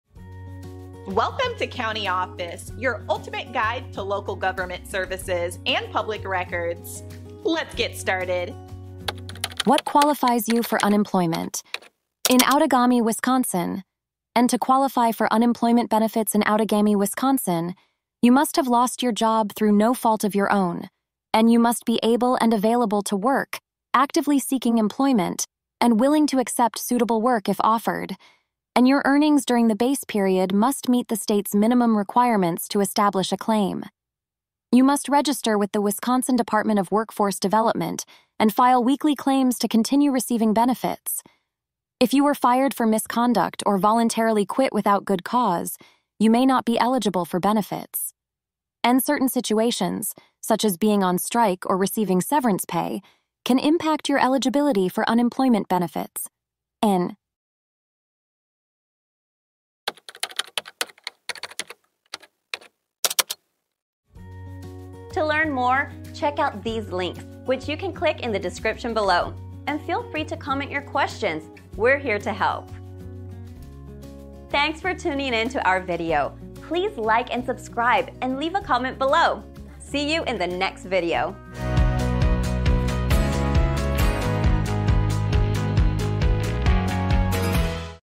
What Qualifies You For Unemployment In Outagamie, Wisconsin? - CountyOffice.org
What Qualifies You For Unemployment In Outagamie, Wisconsin? In this informative video, we delve into what qualifies individuals for unemployment benefits in Outagamie, Wisconsin. Understanding the eligibility criteria and requirements is crucial for those navigating the unemployment system. Learn about the essential factors that determine eligibility, such as job loss circumstances, availability for work, and earnings threshold. Discover the steps you need to take, including registration with the Wisconsin Department of Workforce Development and filing weekly claims. Uncover the impact of certain situations like misconduct, voluntary quitting, strikes, or severance pay on your eligibility for benefits. Stay informed to ensure you receive the support you may be entitled to during challenging times.